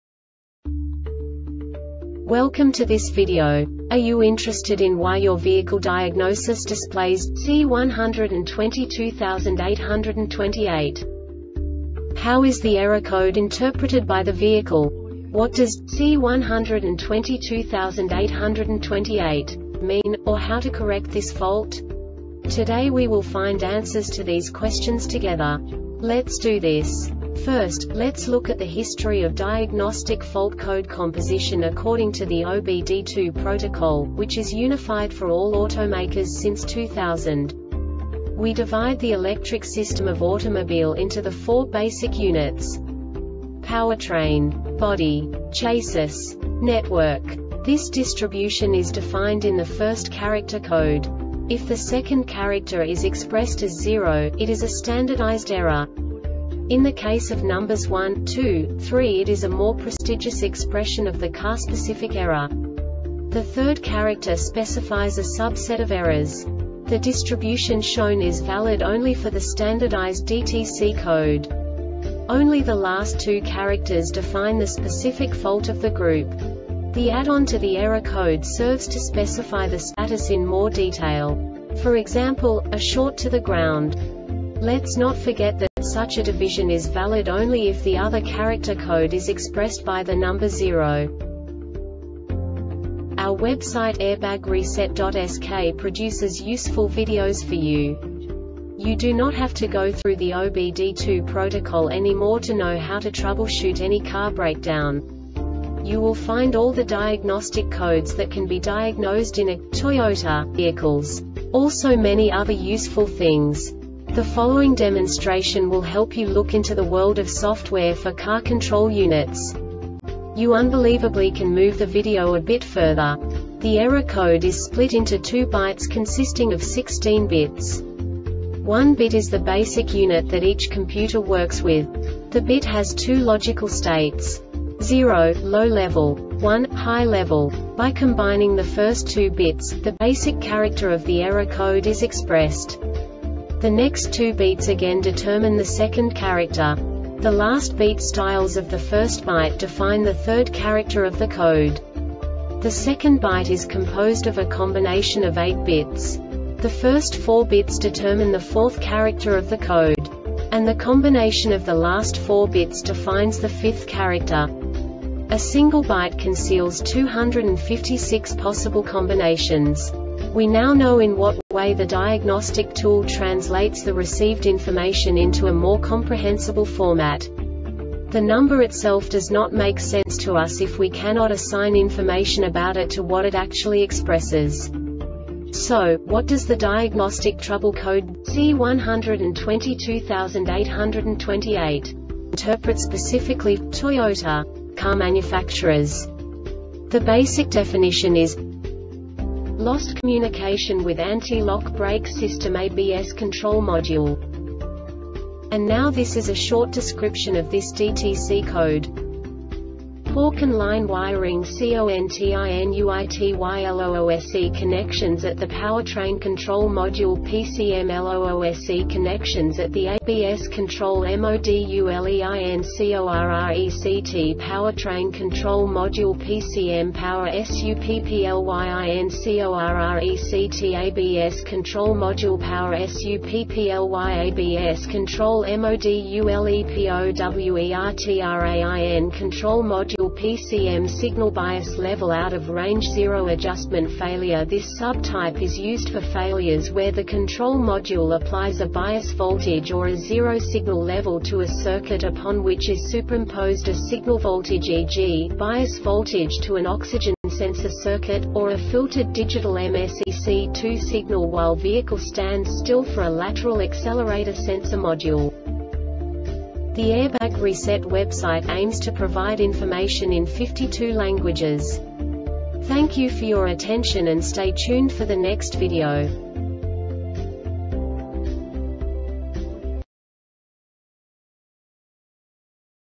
DTC Toyota C1228-28 Short Explanation
Contents:
0:21 Basic DTC analysis according to OBD2 protocol standard.
1:48 Insight into programming
2:58 A diagnostically understandable description of the Diagnostic Trouble Code
3:05 Basic error definition

Make:
Toyota
Code:
C1228 28
Definition:
Cause:
POOR CAN LINE WIRING CONTINUITYLOOSE CONNECTIONS AT THE POWERTRAIN CONTROL MODULE (PCM)LOOSE CONNECTIONS AT THE ABS CONTROL MODULEINCORRECT POWERTRAIN CONTROL MODULE (PCM) POWER SUPPLYINCORRECT ABS CONTROL MODULE POWER SUPPLYABS CONTROL MODULEPOWERTRAIN CONTROL MODULE (PCM)
Failure Type:
Signal Bias Level Out of RangeorZero Adjustment Failure
This sub type is used for failures where the control module applies a bias voltage or a zero signal level to a circuit upon which is superimposed a signal voltage (e.g., bias voltage to an Oxygen Sensor circuit, or a filtered digital m or sec2 signal while vehicle stands still for a lateral accelerator sensor module.)